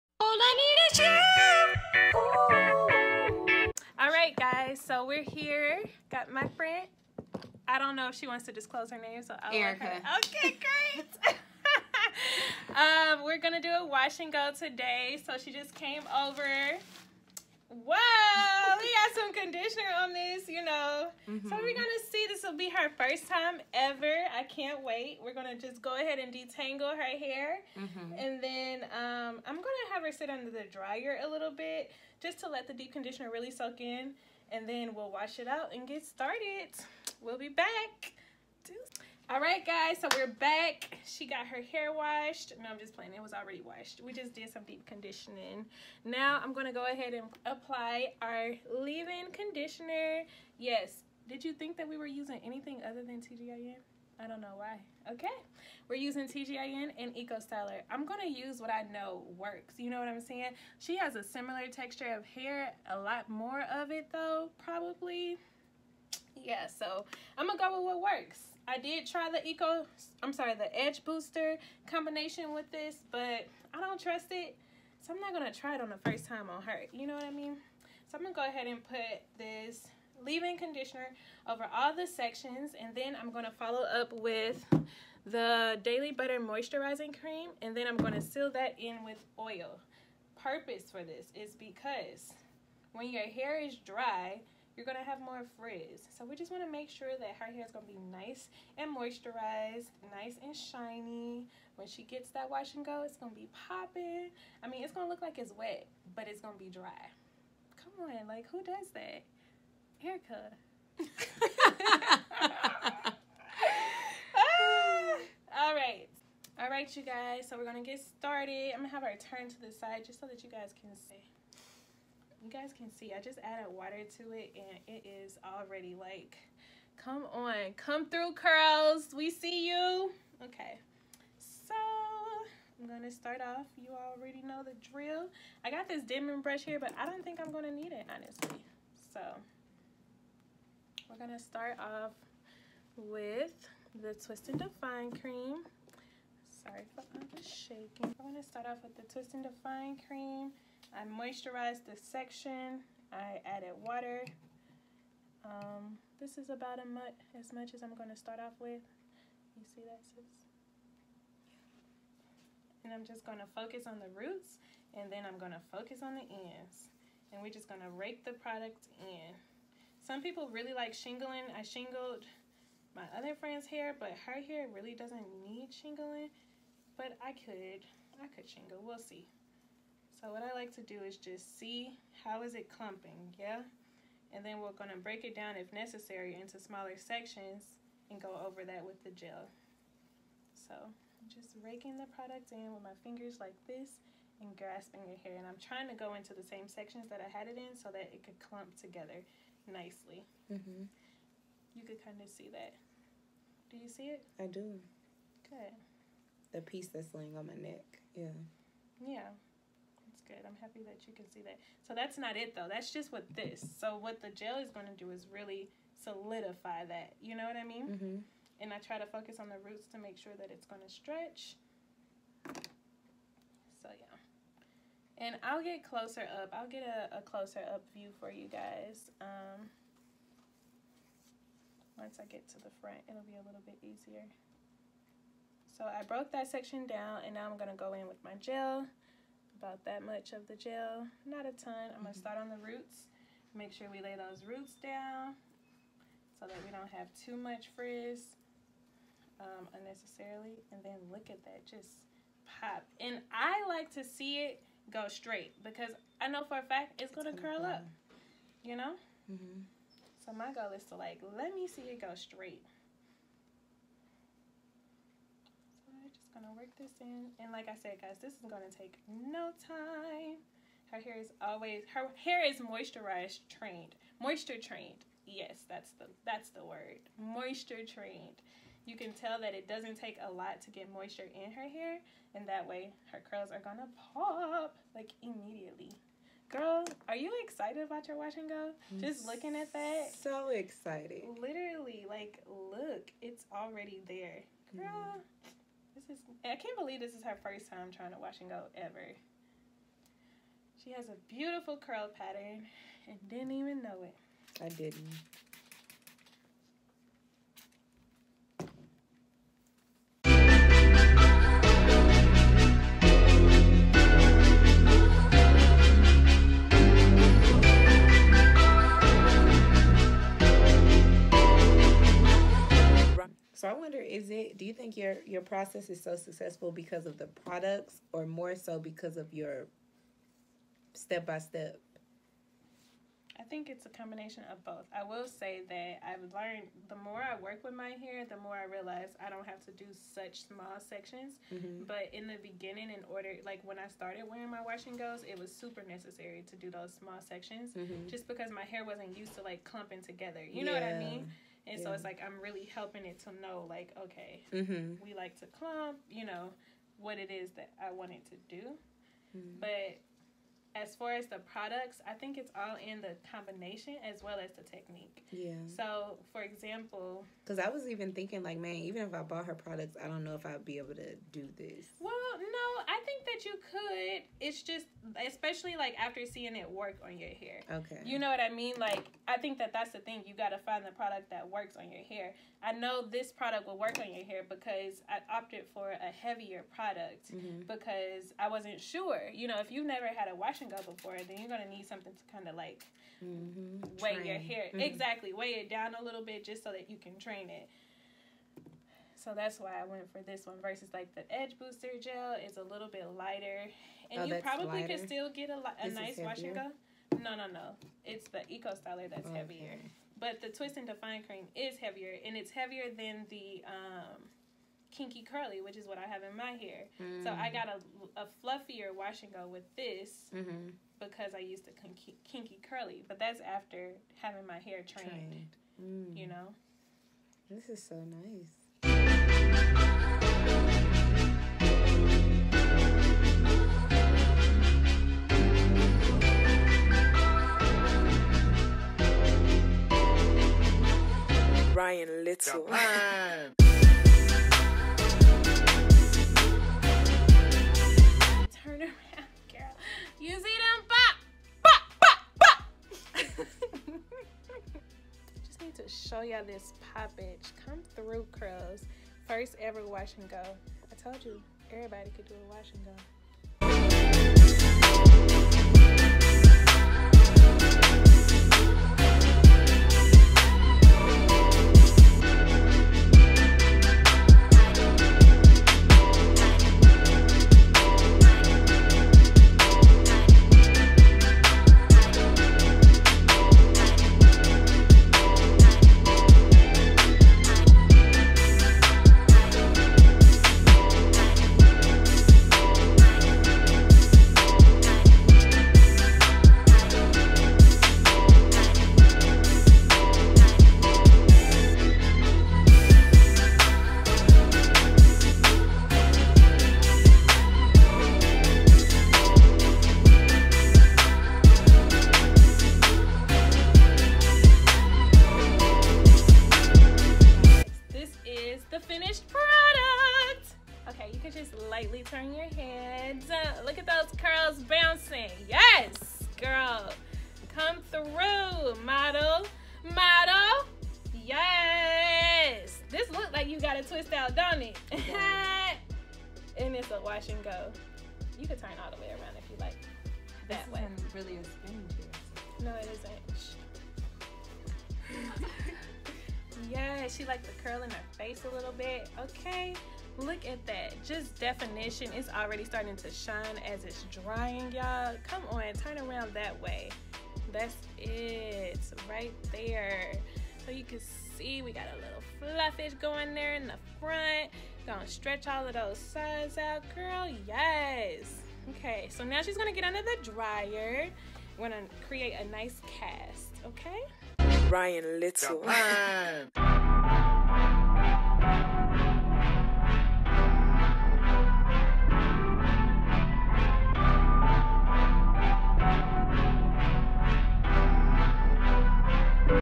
First Time EVER WASH & GO on KINKY Natural Hair feat
Today, I have a GUEST!! This is Erika's first time EVER having a Wash & Go. She was convinced to believe her Natural Hair wasn't the Wash & go type! After seeing these results, who can continue believing such a lie!?!?

For this Wash & Go we used my favorite tried and true combination, TGIN (Thank God It's Natural) thankgoditsnatural and Olive Oil Eco Styler gel. Erika typically styles her hair in twist outs, when she isn't protective styling. However, after THIS Wash & Go, she is going to try adding this style into her rotation!